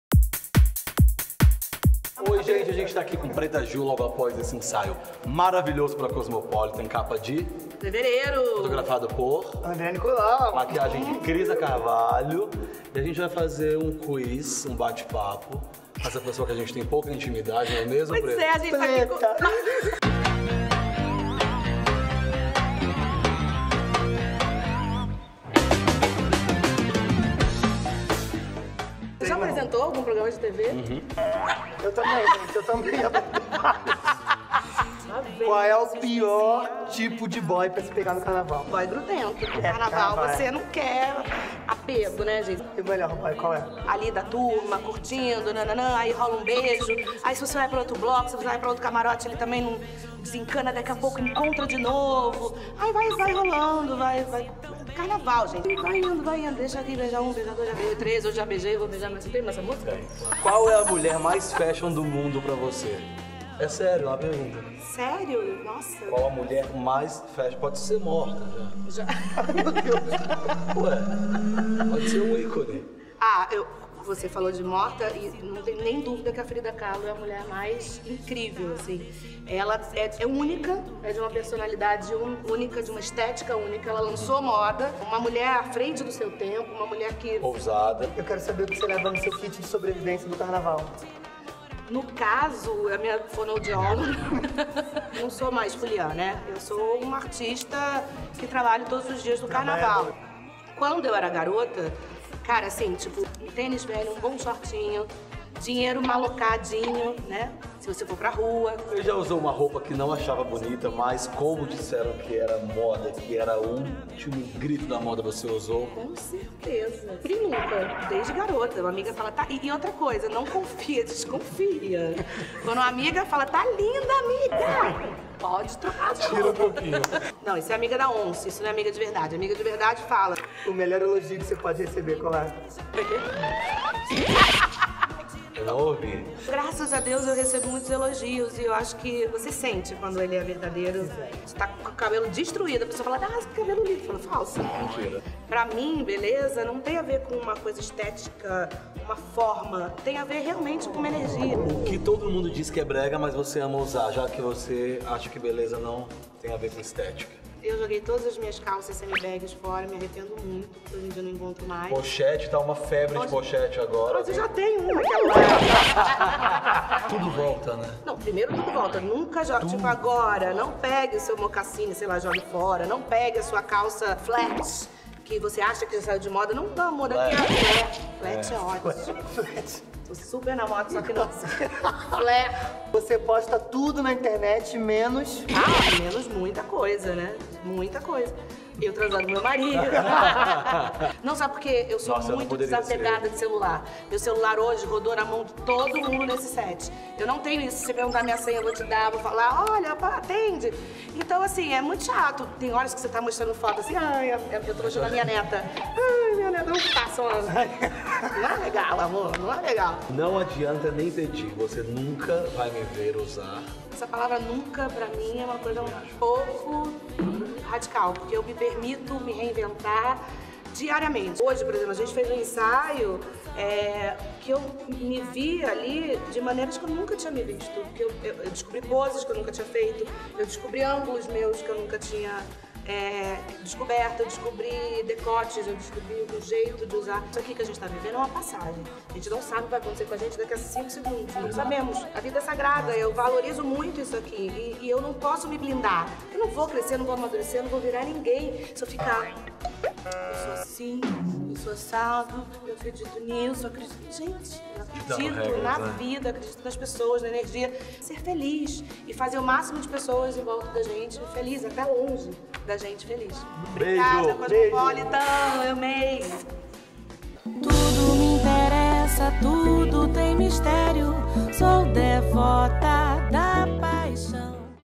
Yan Acioli e André Nicolau entrevistam Preta Gil
O stylist e o fotógrafo responsáveis pelo ensaio de capa de fevereiro de COSMO fizeram perguntas sobre carnaval e moda. Preta falou sobre tudo com aquele bom humor que é só seu.